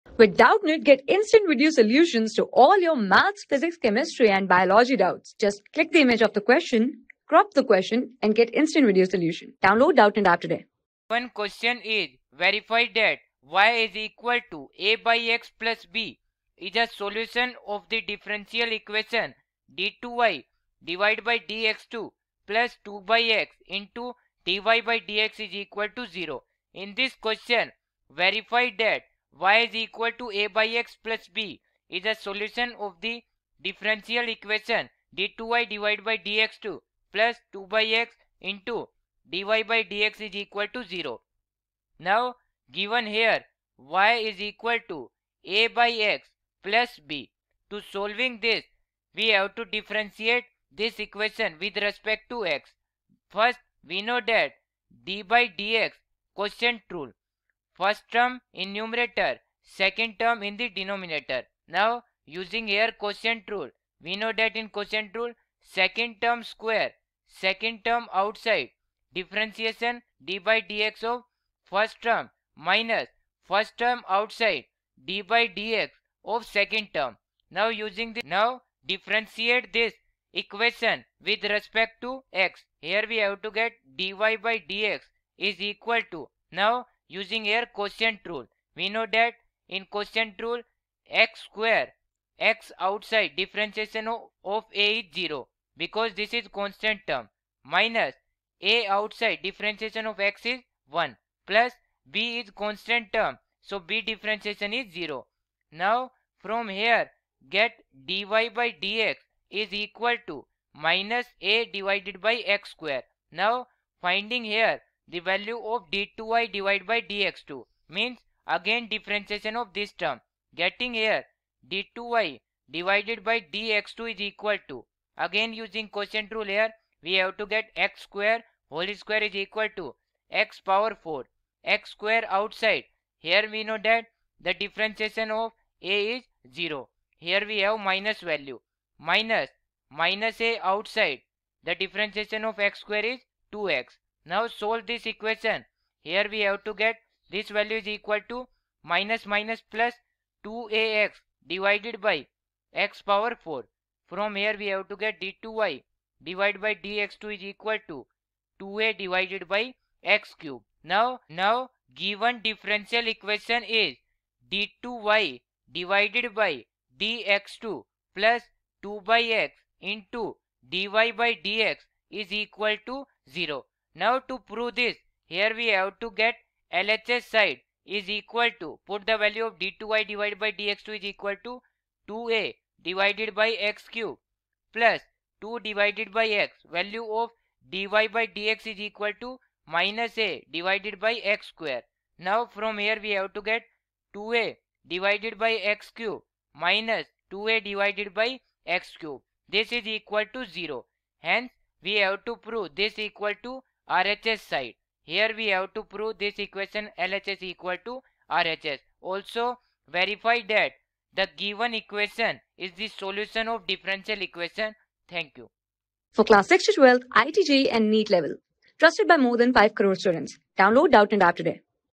Verity that y=a/x+b\nis a solution of the differential equation (d^2y)/(dx^2)+2/x((dy)/(dx))=0. ...
Class: 12
Subject: MATHS
Chapter: DIFFERENTIAL EQUATION
Board:CBSE

👉 Have Any Query? Ask Us.